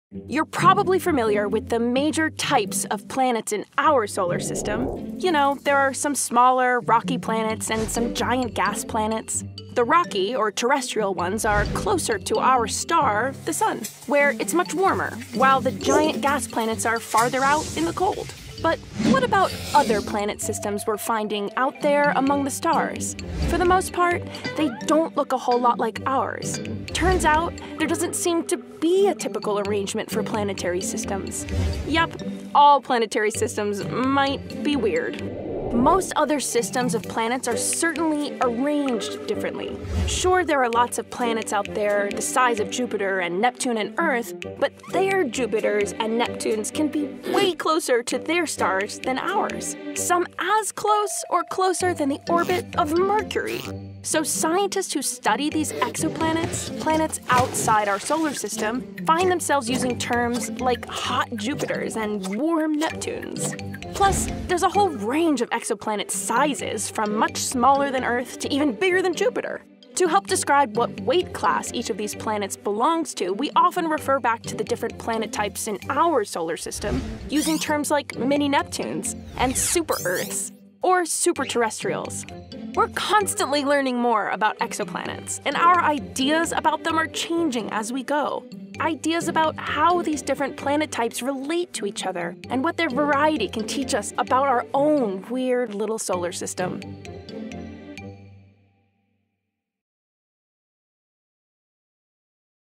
Exoplanet Types： Worlds Beyond Our Solar System
When we describe different types of exoplanets – planets outside our solar system – what do we mean by "hot Jupiters," "warm Neptunes," and "super-Earths"? Since we're still surveying and learning about the variety of worlds out there among the stars, it's sometimes helpful to refer to characteristics they share with planets we're familiar with in our own planetary system.
Credit: NASA/JPL-Caltech